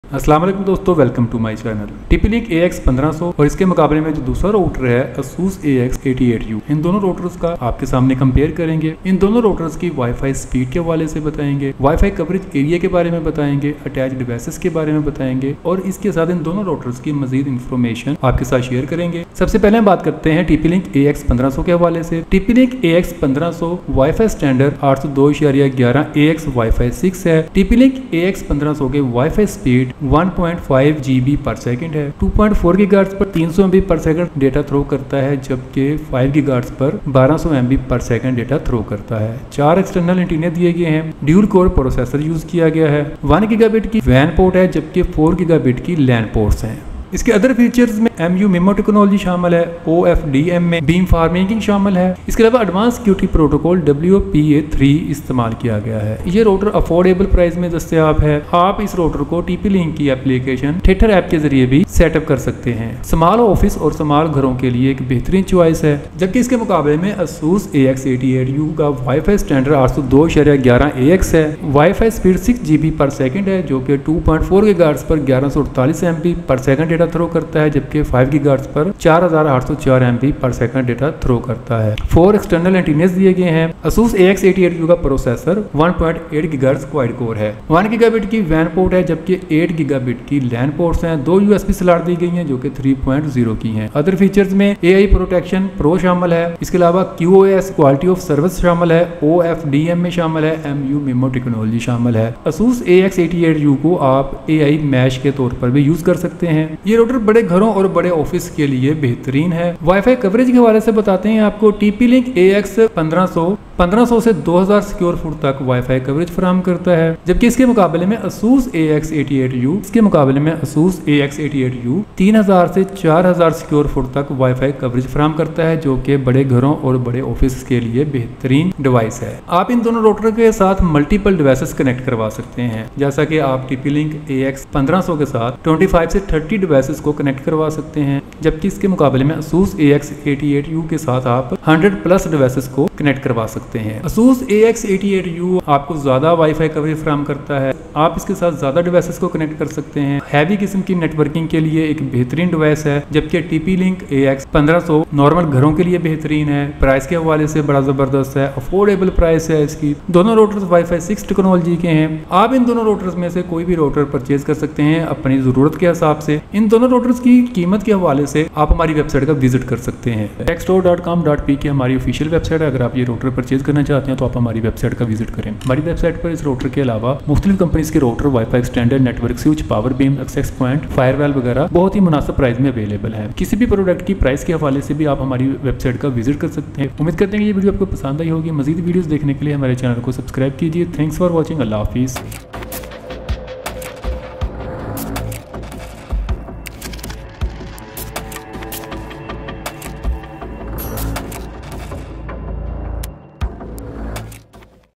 TP-Link AX1500 vs ASUS AX88U | Speed, Coverage, and Features
In this video, we’re diving deep into a comparison between two popular Wi-Fi 6 routers: the TP-Link AX1500 and the ASUS AX88U. We’ll explore their key features, performance, coverage, and suitability for different types of users. Whether you’re looking for a budget-friendly router for a small home or a high-performance device for a large household, we’ve got you covered.

What You’ll Learn:
Coverage Area: How many square feet each router can cover
Device Capacity: The number of devices each router can handle efficiently
Speed and Performance: Real-world speed tests and performance insights
Features Comparison: Key features that set these routers apart
Best Use Cases: Which router is best for your specific needs